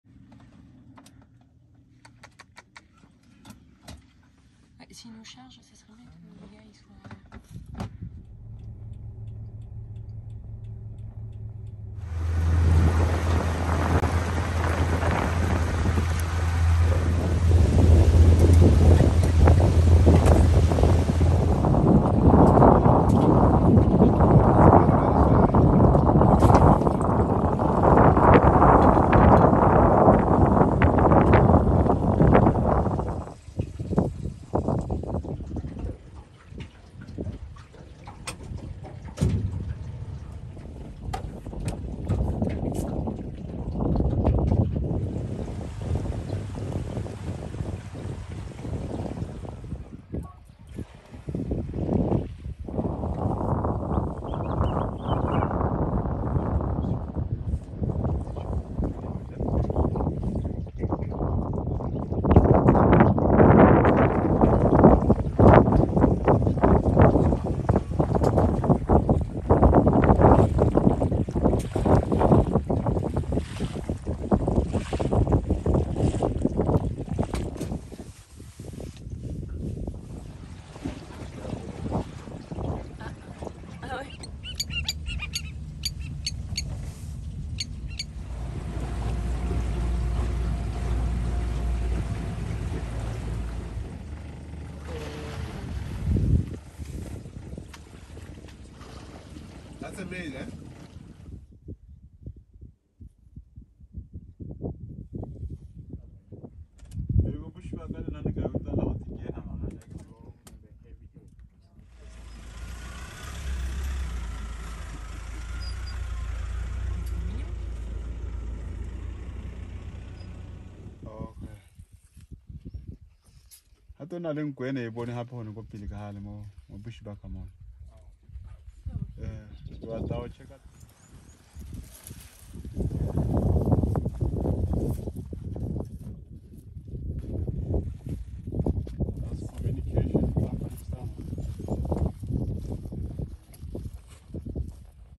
Sights and Sounds of Chobe National Park
Some of the animals discovered at Chobe National Park in Botswana.